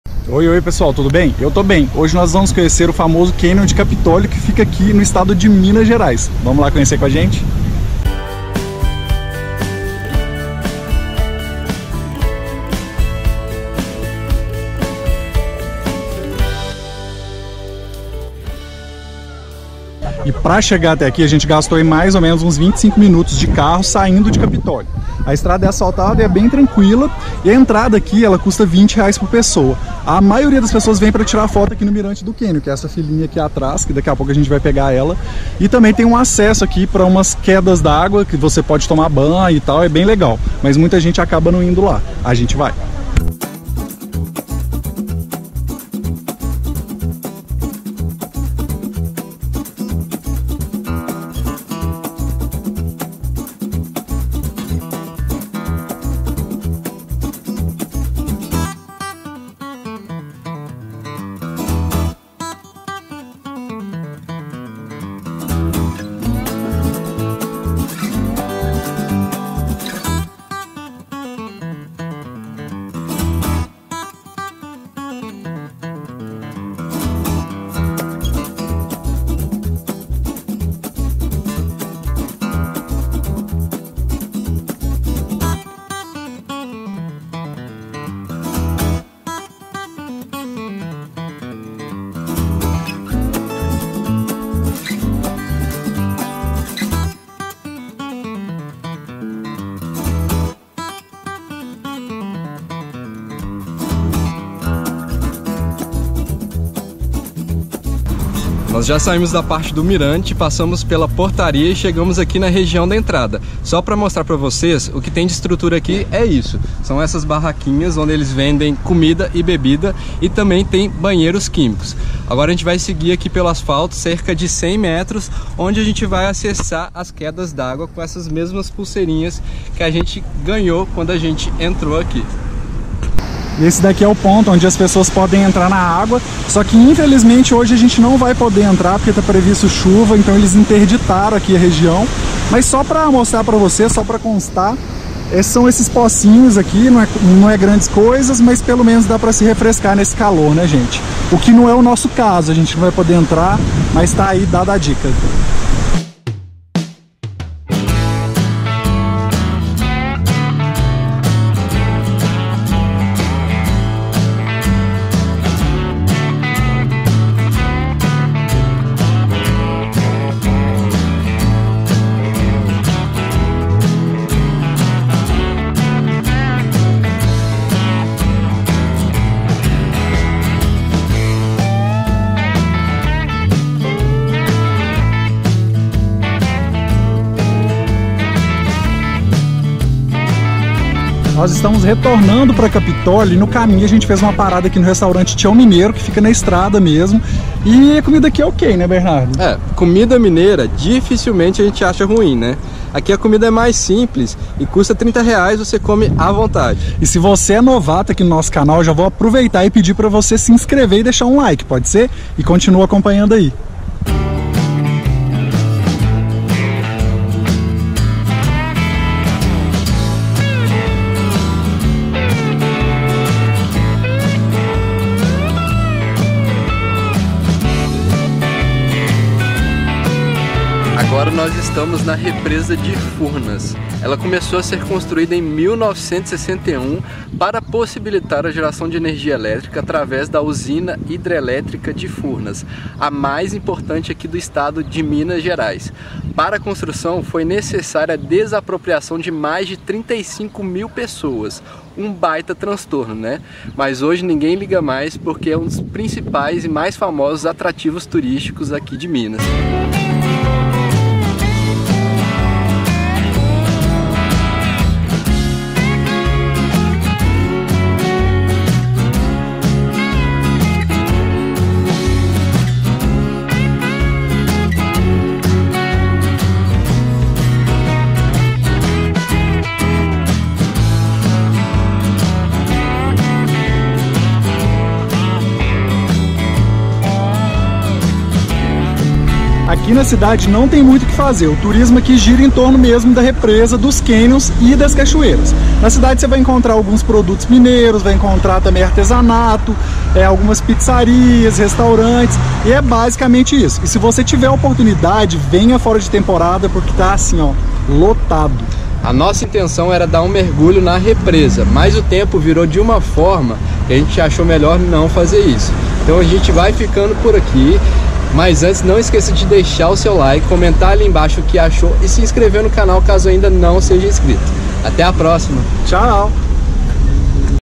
Mirante dos Canyons | Capitólio | Drone
Neste vídeo você verá um dos lugares mais lindos de Minas Gerais: os canyons, que ficam entre Capitólio e São João Batista do Glória. Fizemos lindas imagens (de drone), e também demos dicas de onde comer e mostrar um pouquinho da cidade.
Esperamos que goste!
I
nscreva-se em nosso canal:

Em Capitólio ficamos hospedados em uma casa (alugamos um quarto), há cerca de 5 minutos do centro da cidade (de carro). Pagamos um pouco mais de 100 reais a diária (para o casal).